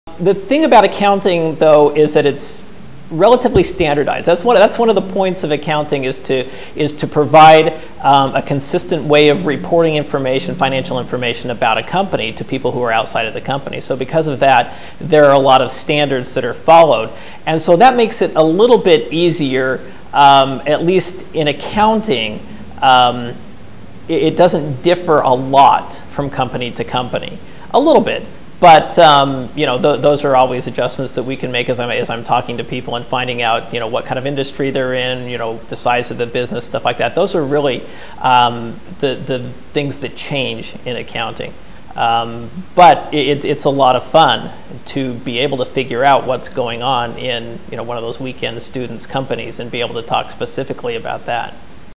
Krannert School of Management MS(A) - The Consistency of Accounting
Krannert Professor Troy Janes talks about the consistency of accounting throughout different industries.